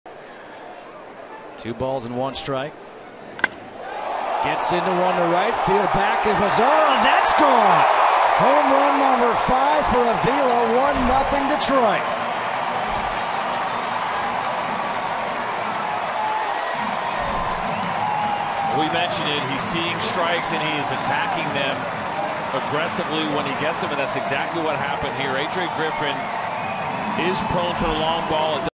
TEX@DET: Avila's solo shot opens the scoring
Alex Avila hits his fifth home run of the season, a solo jack to right, to give the Tigers a 1-0 lead in the bottom of the 1st